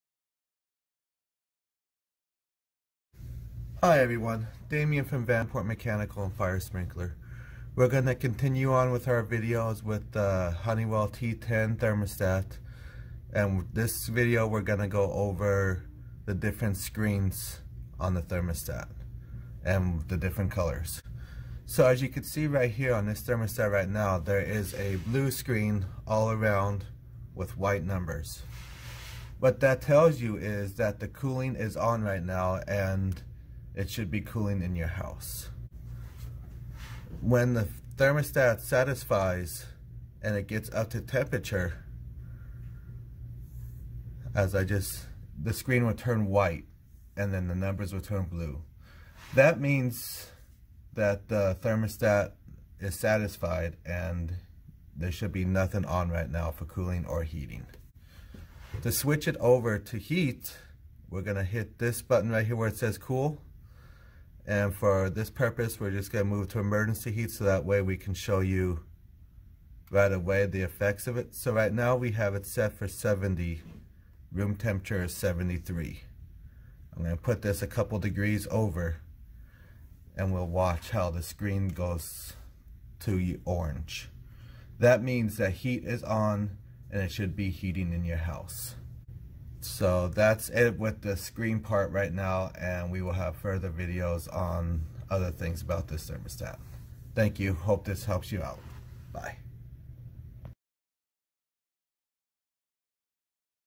Honeywell T10 Pro Smart Home Thermostat Screen Colors
Damian from Vanport Mechanical and Fire Sprinkler, Inc. shows you some of the features of the Honeywell T10 Pro Smart Home thermostat, including how the screen can be set to change colors based on whether your system is in heating or cooling mode.

for all your HVAC and Fire Suppression System needs!